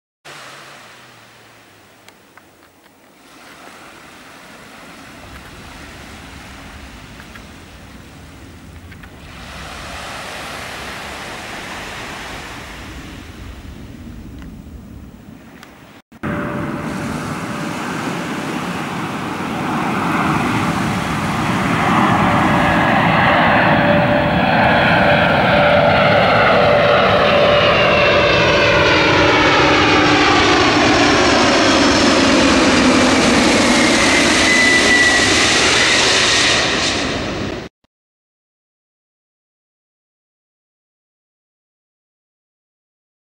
Antonov 124-100 cargo plane
Antonov cargo plane flying over Glenelg Beach, then landing at Adelaide Airport, 12 December 2017